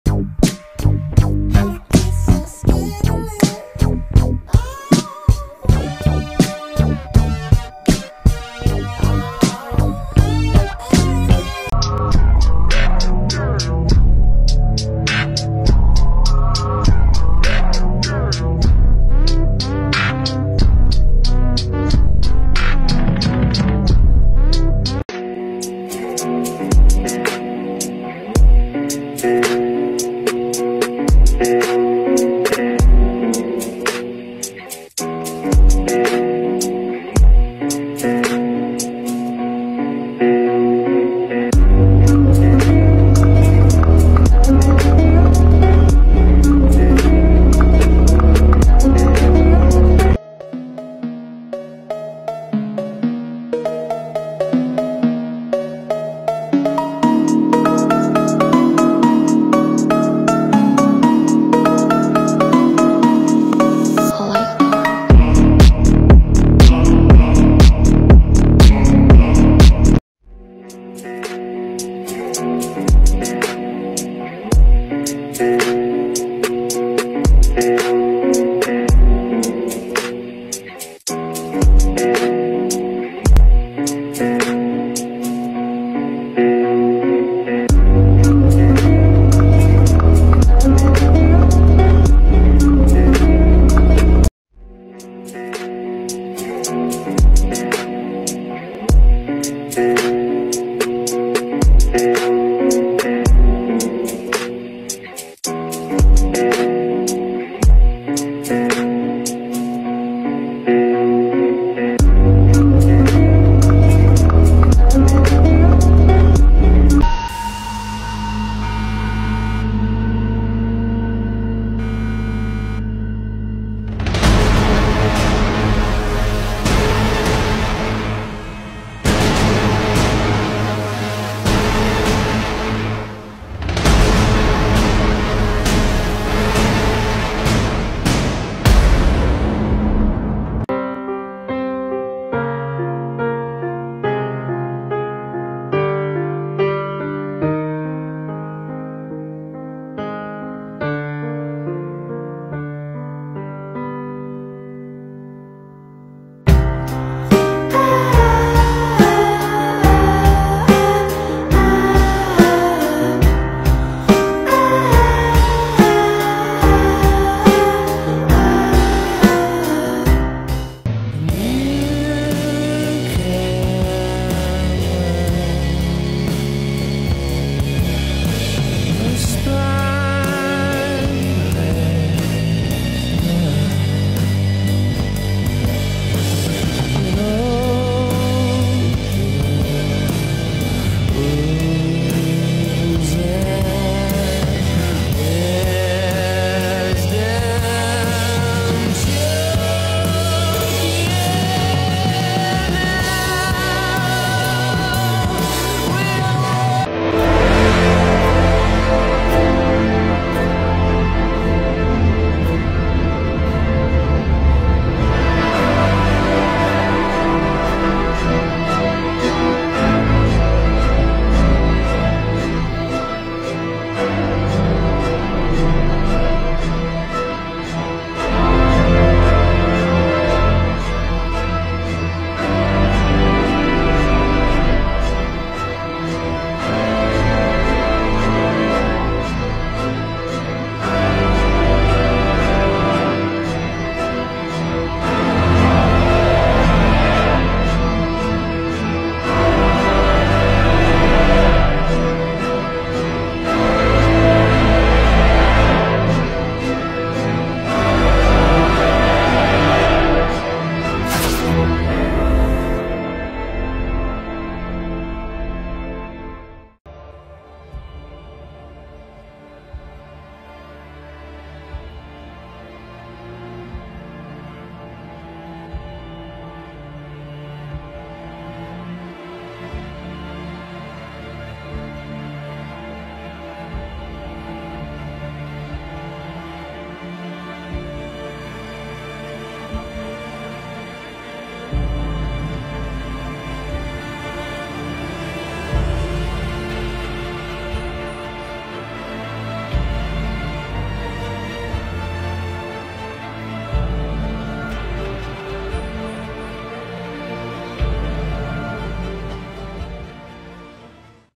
Grus Redemption Arc (TikTok Briman2000) FULL SERIES Part 1-11
the infamous despicable me parody series that started from a trend and turned into something bigger.